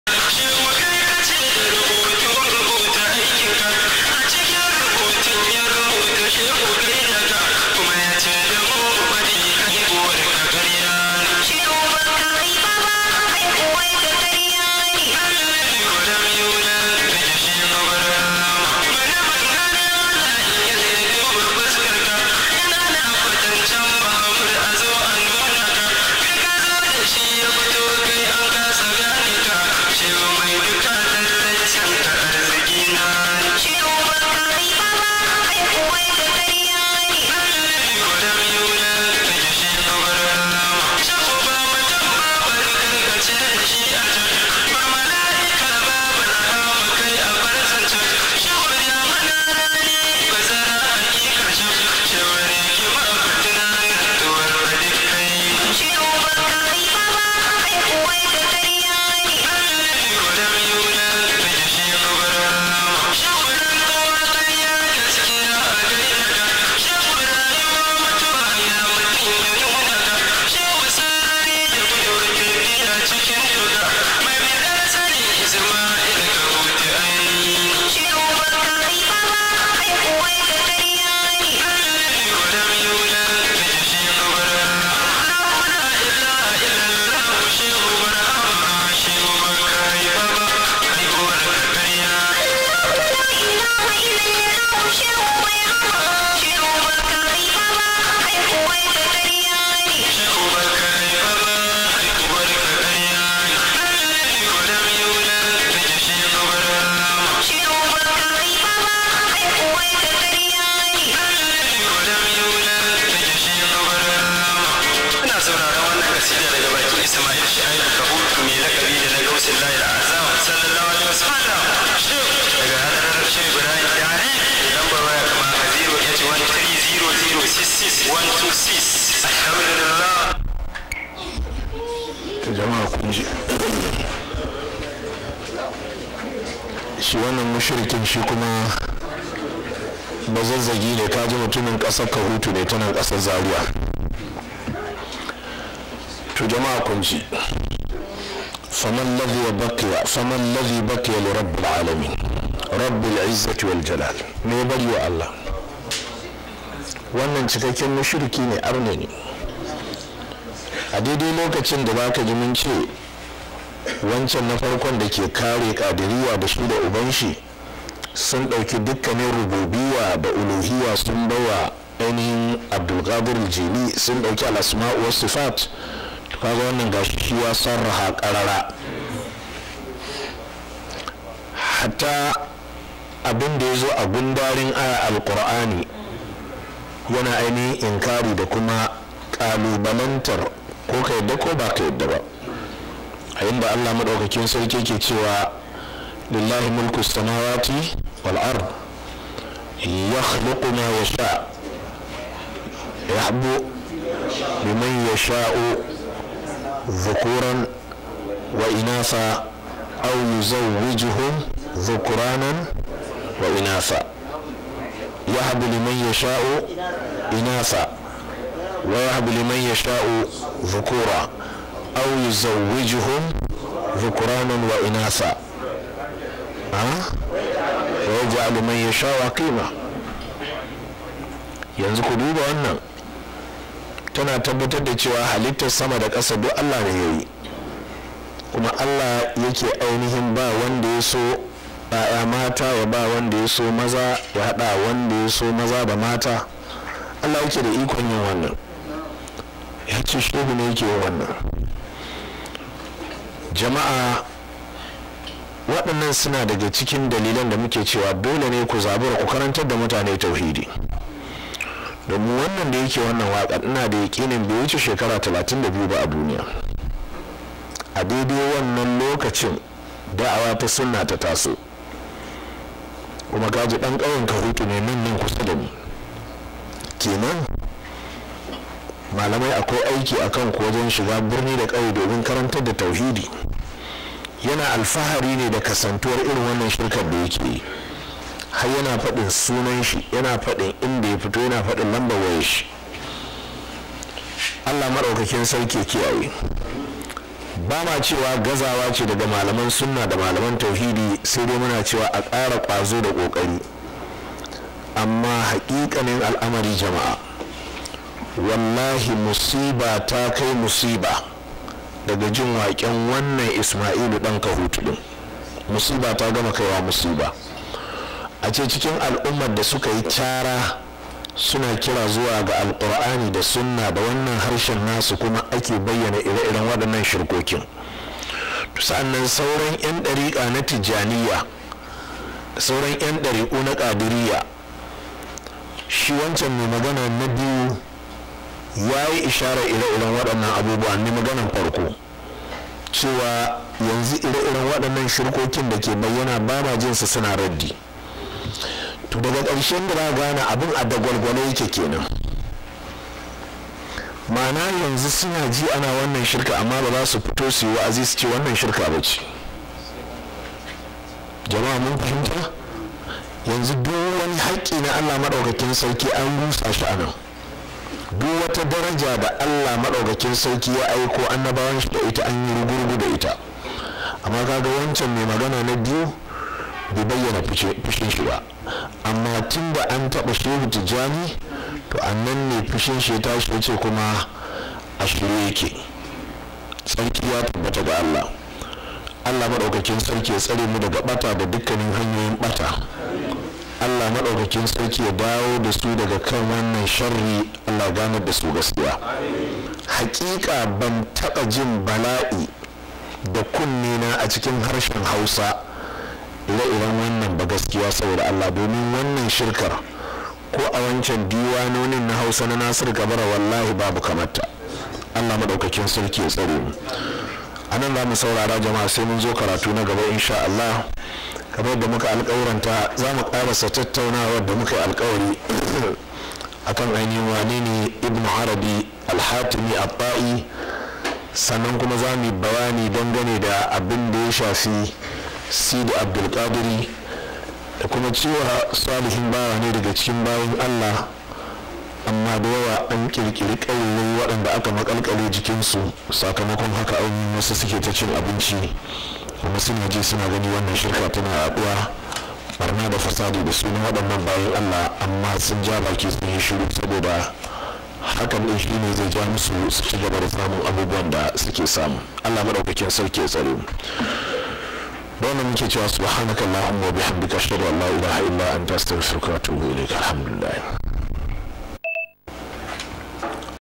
Hamawiyyah 63-64(4/4)/91: Albani
Karatun littafin Fatwa Al-Hamawiyyah Al-Kubra wallapan Shaikhul Islam Ibn Taimiya.

Marketed & distributed by: Daarul-Hadeethis-Salafiyyah (DHSN) Zaria, Kaduna State, Nigeria. Copyright reserved.